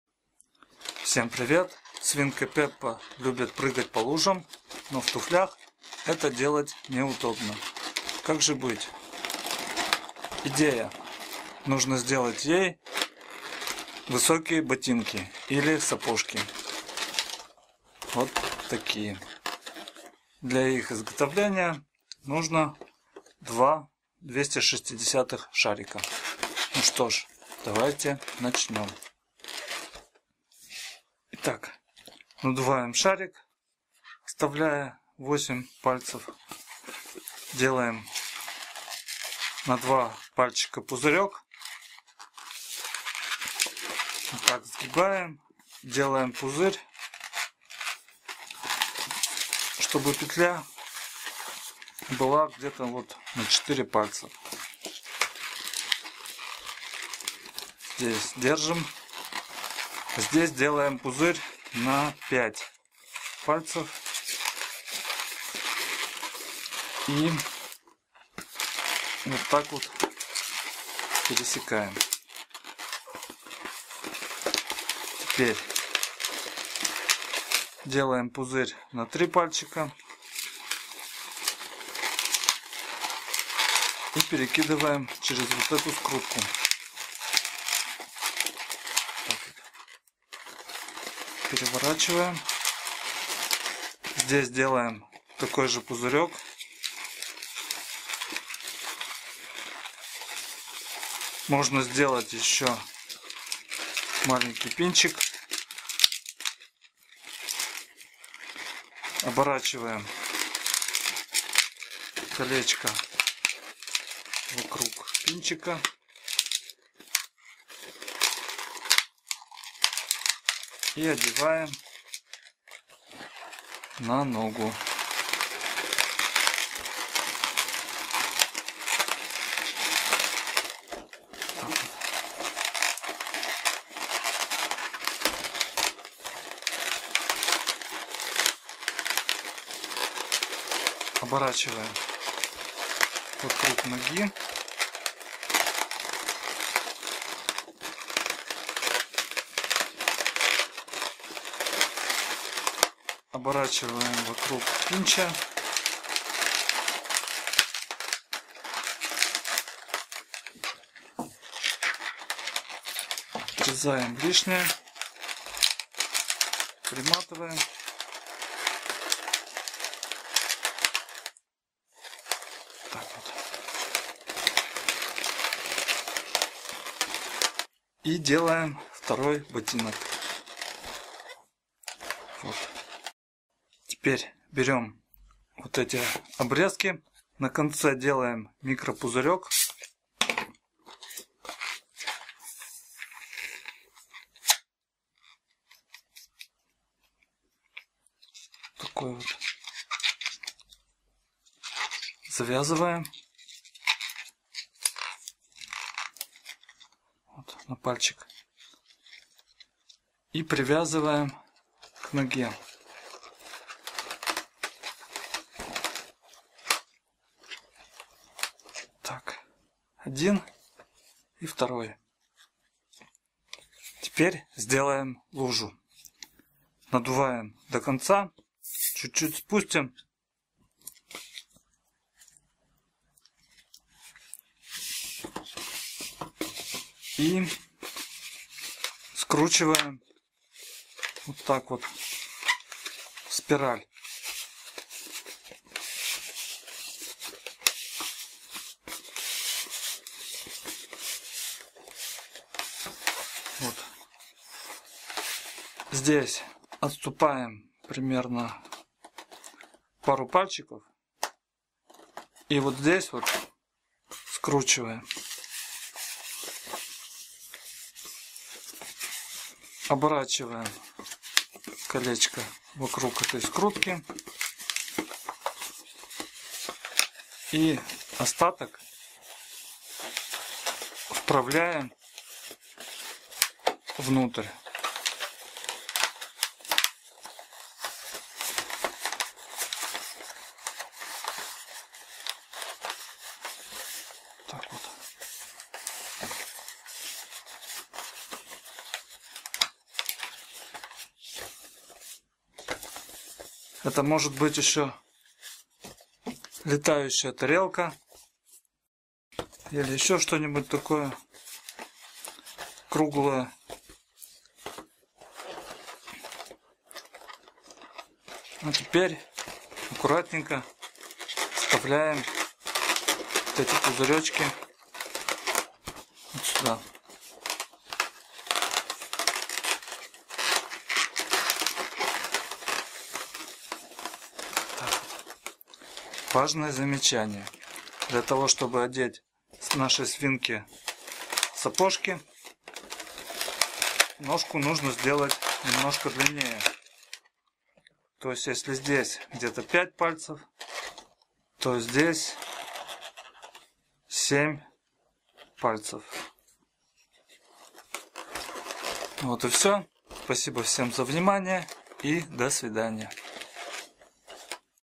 Ботинки и лужа для Пеппы / Boots and puddle for Peppa (Subtitles)
Как сделать ботинки (сапожки) и лужу для свинки Пеппы из шаров ШДМ.
How to make a boots and puddle for Peppa pig of balloons - twisting tutorial. Any comments about subtitles are welcomed :)
Designed by Sergey Loginov